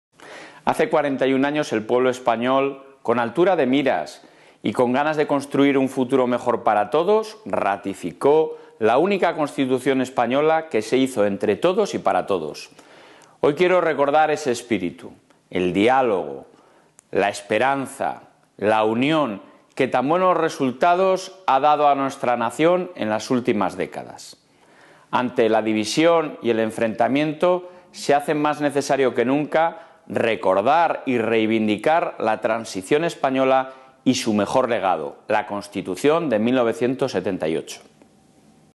Mañueco reivindica el "espíritu" de entendimiento de la Transición
El presidente de la Junta de Castilla y León, Alfonso Fernández Mañueco, ha reivindicado el "espíritu de la Transición" española y de un "paisano" de Castilla y León como Adolfo Suárez, que encarnó el "entendimiento, el diálogo y la búsqueda de consenso entre las grandes fuerzas políticas", algo que, a su juicio, es "fundamental".